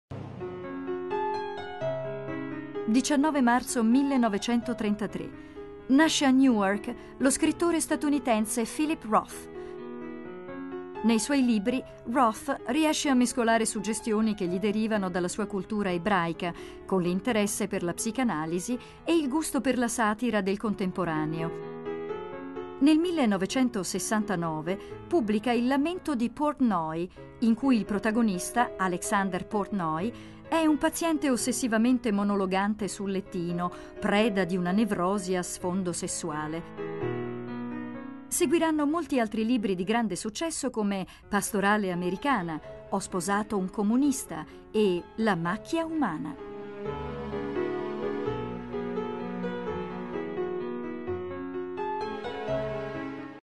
19 marzo 1933 nasce Philip Roth
Nasce a Newark lo scrittore statunitense Philip Roth. Nei suoi libri, Roth riesce a mescolare suggestioni che gli derivano dalla sua cultura ebraica con l’interesse per la psicanalisi e il gusto per la satira. Nel 1969, pubblica “Lamento di Portnoy”, in cui il protagonista Alexander Portnoy, un paziente ossessivamente monologante sul lettino, e’ preda di una nevrosi a sfondo sessuale. Seguiranno molti altri libri di grande successo come “Pastorale americana”, “Ho sposato un comunista” e “La macchia umana”.